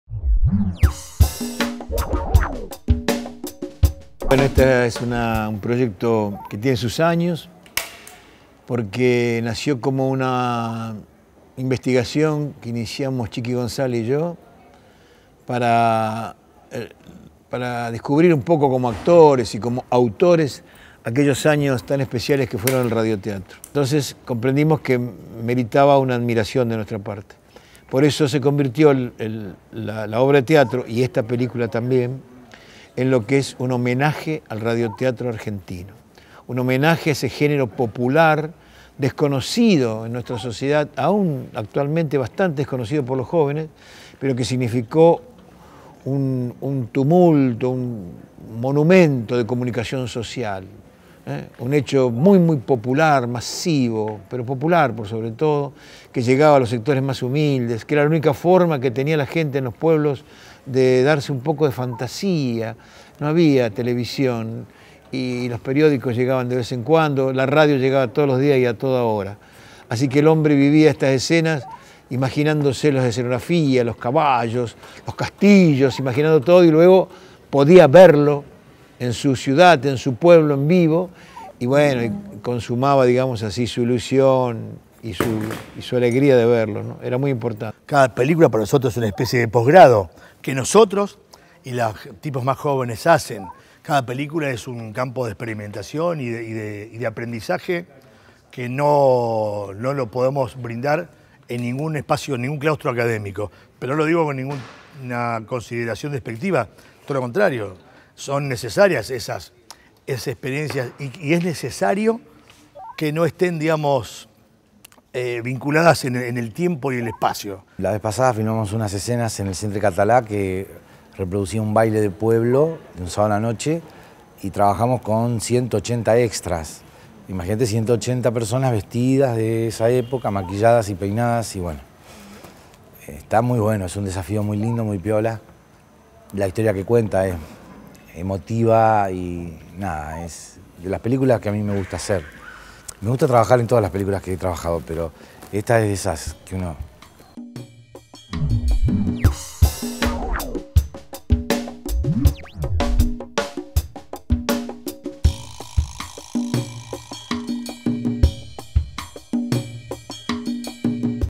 Bienvenido León de Francia, una película de Néstor Zapata
Visitamos el rodaje de la película que dirige Néstor Zapata, docente de nuestra Universidad.  En su producción, 100% rosarina, participan también egresados de la Escuela de Comunicación Social.